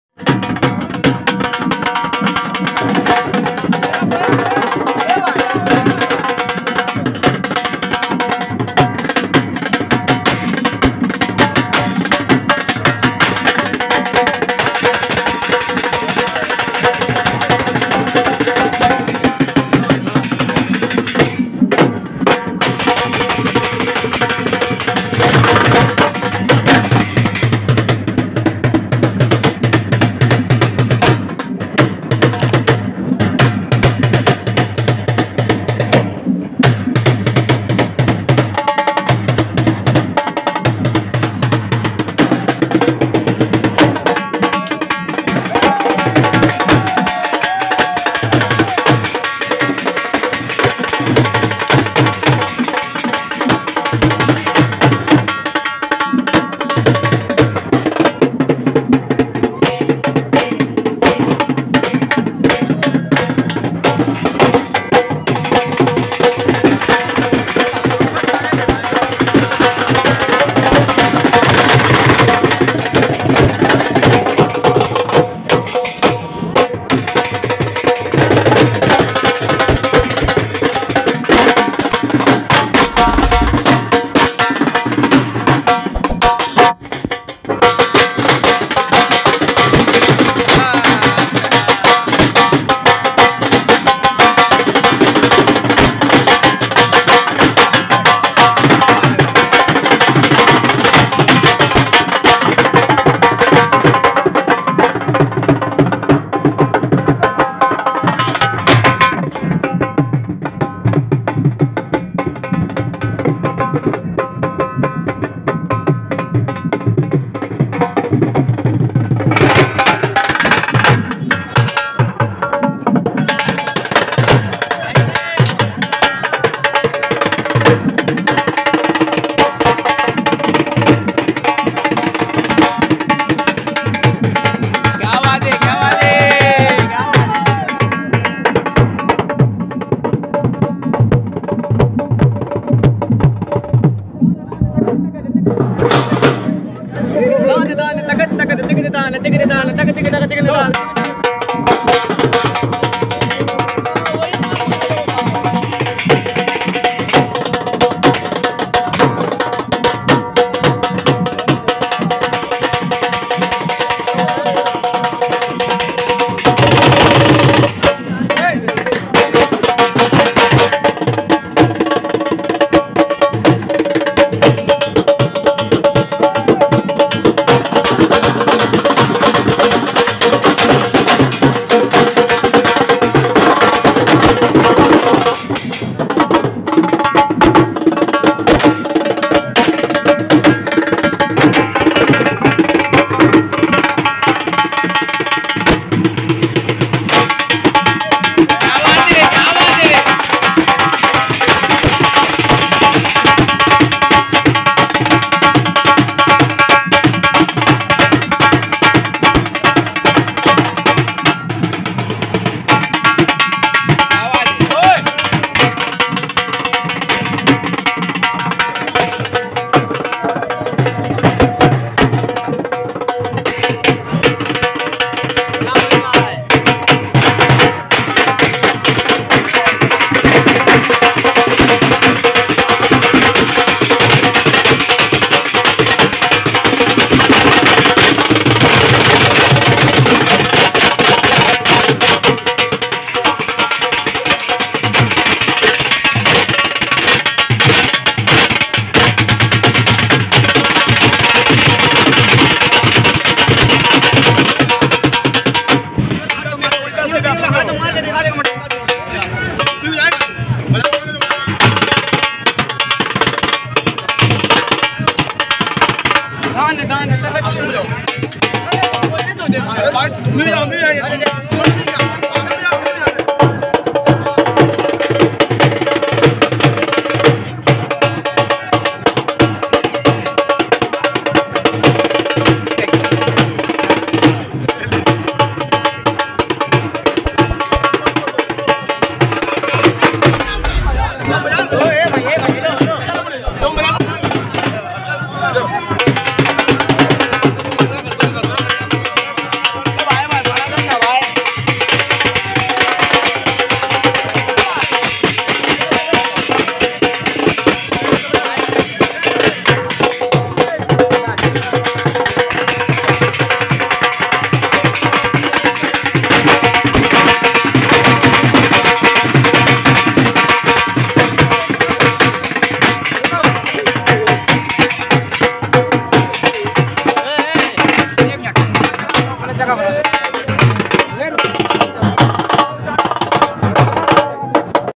Dipak Vs Kanpur Dhol Players | Kolkata Chhat Puja 2023 | Dhol Jugalbandi
Dipak Vs Kanpur Dhol Players | Kolkata Chhat Puja 2023

Thanks To All The Club Members For Giving Me This Opportunity ☺️

Your Subscription Can Make My Day 😉